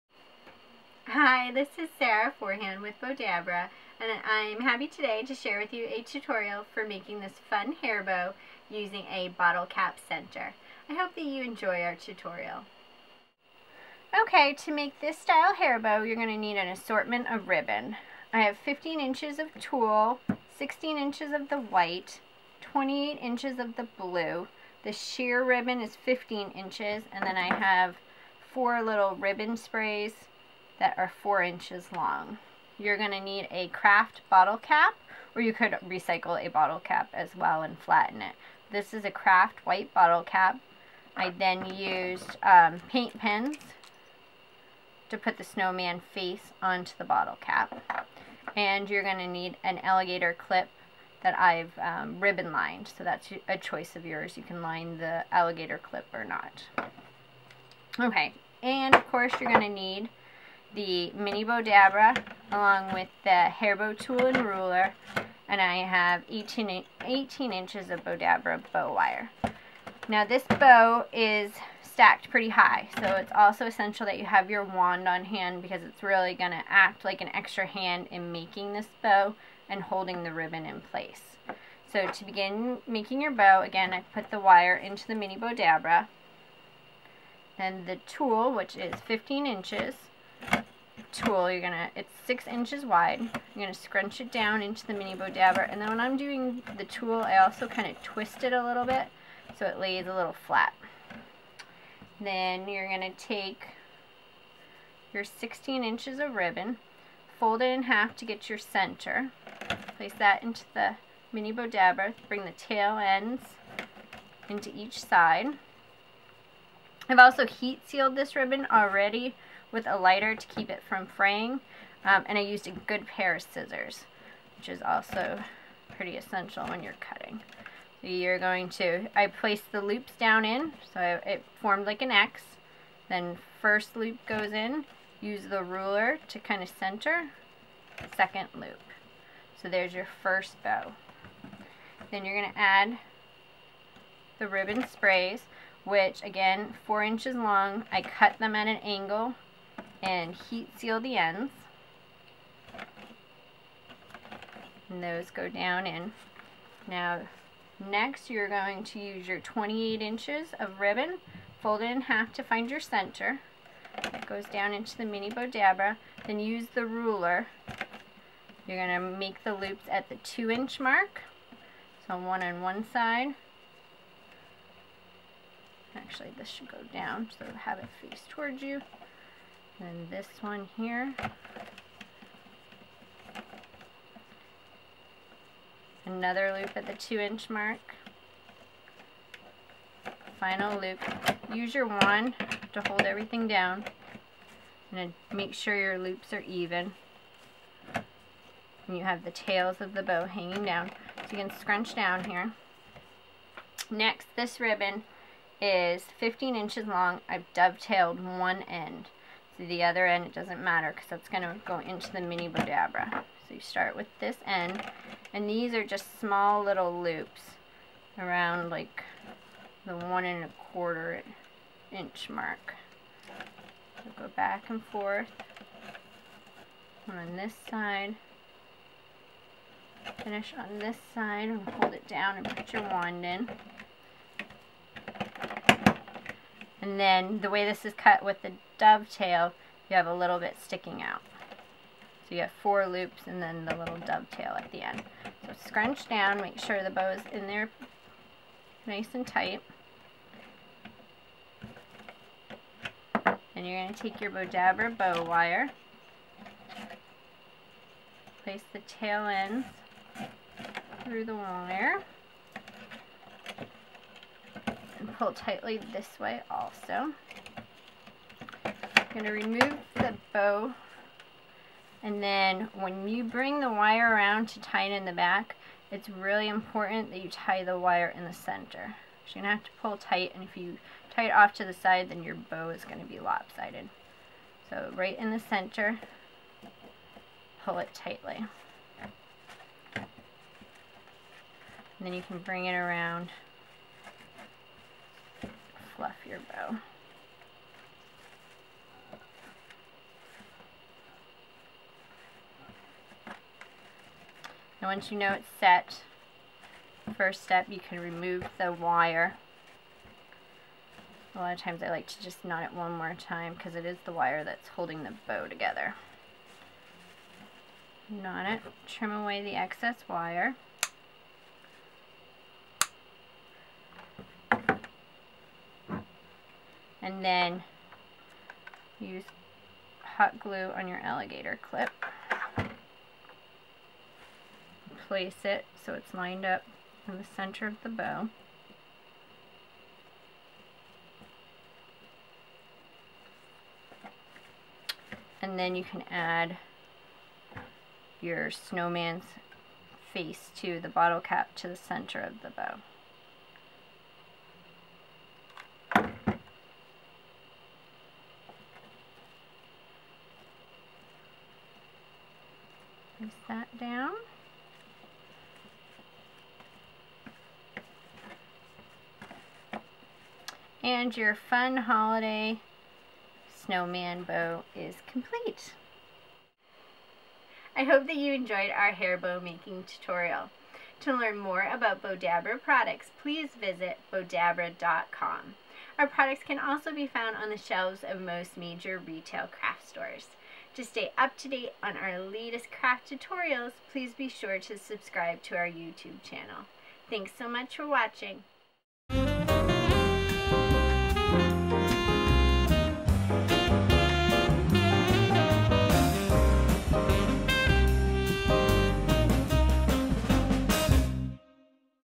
Bowdabra Snowman Bottle Cap Hair Bow
Use the Mini Bowdabra to make two matching snowman bottle cap hair bows this holiday season.